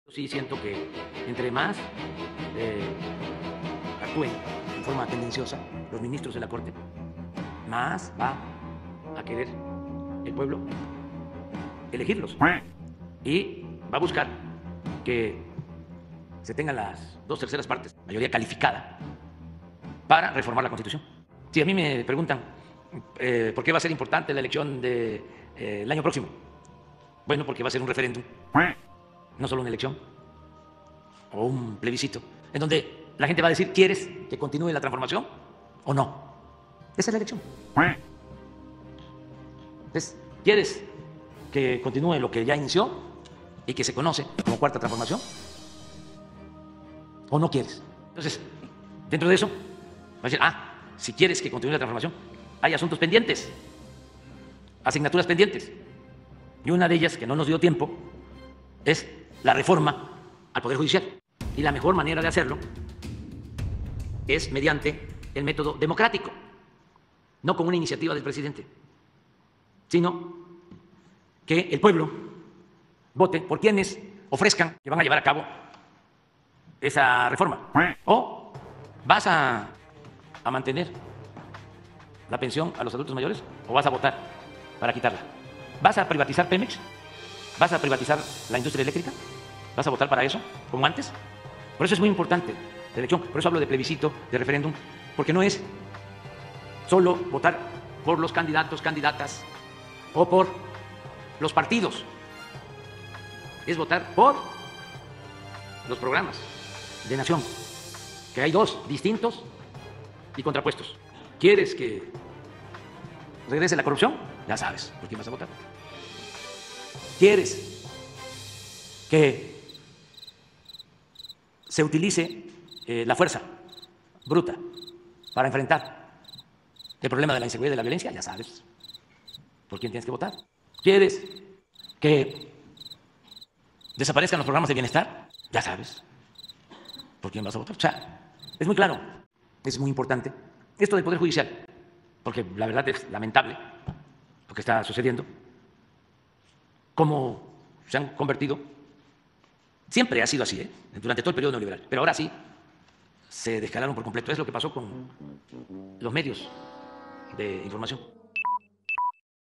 🚨 Insiste que la elección de 2024 es un plebiscito porque se elegirá entre dos proyectos de nación 🦆
🚨 Insiste que la elección de 2024 es un plebiscito porque se elegirá entre dos proyectos de nación y destaca la importancia de ganar la mayoría en las Cámaras para reformar la Constitución 🦆